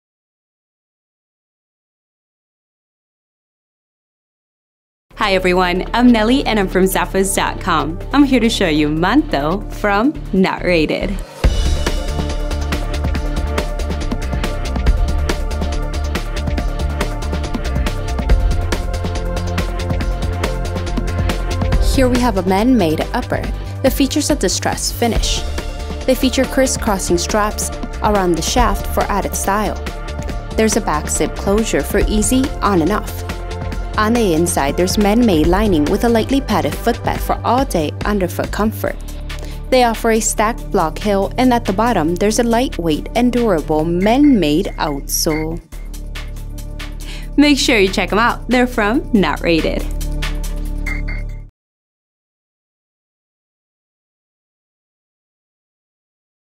Giày bốt Not Rated Manto
Xác định lại bộ sưu tập mùa thu của bạn với chiếc áo khoác Not Rated ™ Manto của người mộc mạc! Vật liệu trên được làm bằng tay nhân tạo có kết thúc đau đớn. Đóng cửa bằng kẹp ở mặt sau.